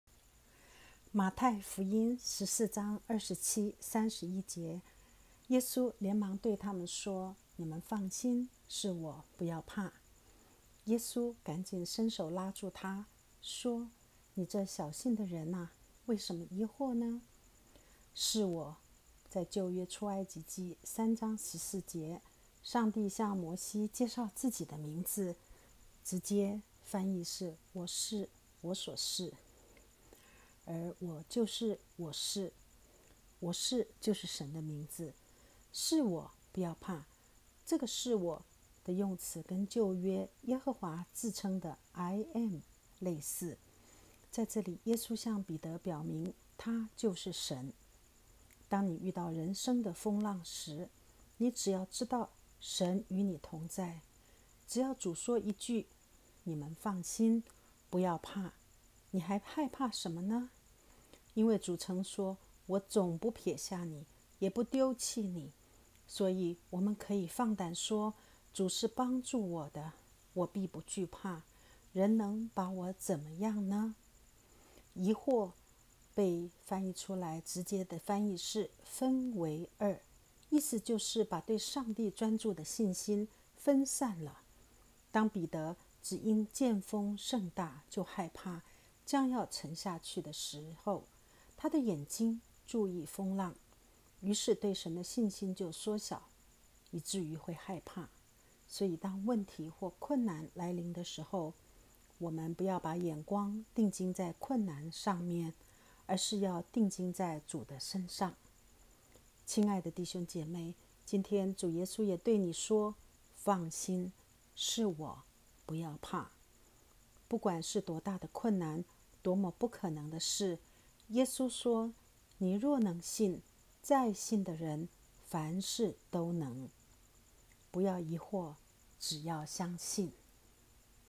馬太福音42
馬太福音14:27,31
耶穌連忙對他們說、你們放心．是我、不要怕。
耶穌趕緊伸手拉住他、說、你這小信的人哪、為甚麼疑惑呢。

●「是我」：舊約出埃及记3:14，上帝向摩西介绍自己的名字，直譯為 「我是我所是」，和合本譯為「我是自有永有的」，KJV 譯為 I AM THAT IAM，我就是「我是」。 「我是」就是神的名字。「是我」，不要怕：這個「是我」的用詞跟舊約耶和華自稱的「I am」類似。在這裏耶穌向彼得表明祂就是神 ！當你遇到人生的風浪時，你只要知道神與你同在，只要主說一句：「你們放心！不要怕！」你還害怕甚麼呢？「因為主曾說：我總不撇下你，也不丟棄你。所以我們可以放膽說：主是幫助我的，我必不懼怕；人能把我怎麼樣呢？」(來13:5-6)。

●「疑惑」：原文是διστάζω，直譯是「分為二」，意思是把對上帝專注的信心分散了。當彼得「只因見風甚大，就害怕，將要沉下去」時，他的眼睛去注意風浪時，於是對神的信心就減小，以致於會害怕了。所以，當問題或困難來臨時，你不要把眼光定睛在困難上，而是要定睛在主的身上！

親愛的弟兄姊妹，今天主耶穌也在對你說：「放心！是我，不要怕！」不管是多大的困難，多麼不可能的事，耶穌說：「你若能信，在信的人，凡事都能。」（可9:23）不要疑惑，只要相信！